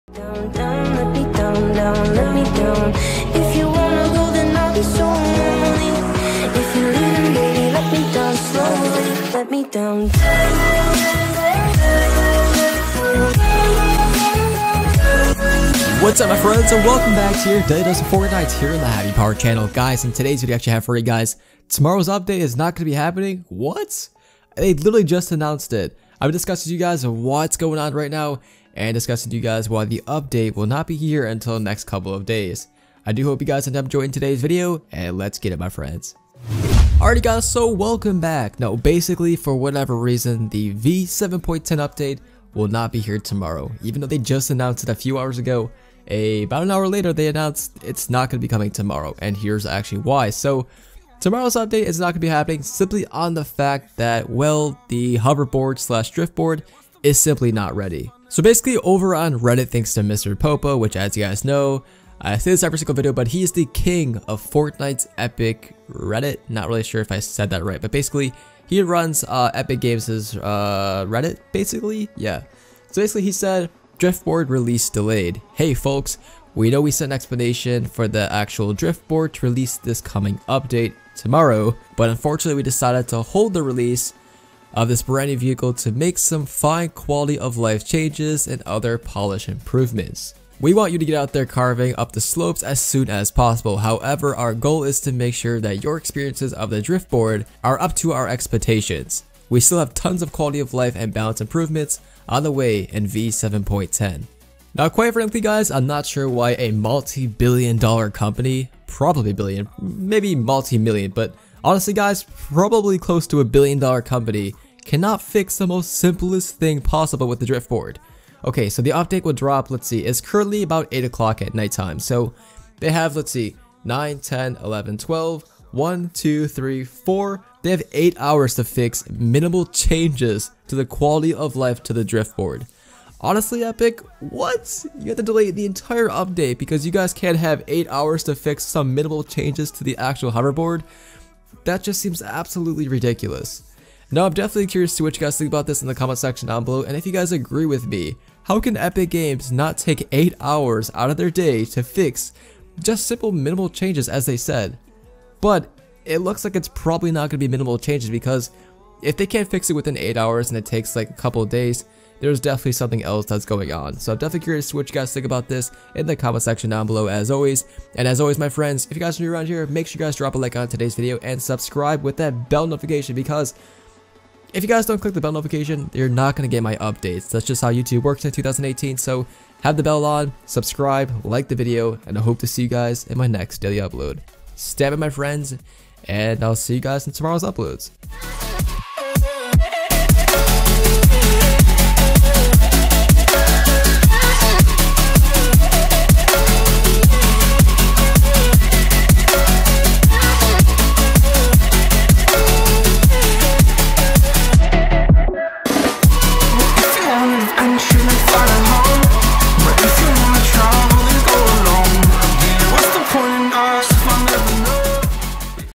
*NEW* Fortnite Update! Tomorrows Update DELAYED! *Driftboard Issues* (v7.10 Delay)
Today's Video: *NEW* Fortnite Update! Tomorrows Update DELAYED! *Driftboard Issues* (v7.10 Delay). Enjoyed this video?

GAMERTAG: Happiness In HD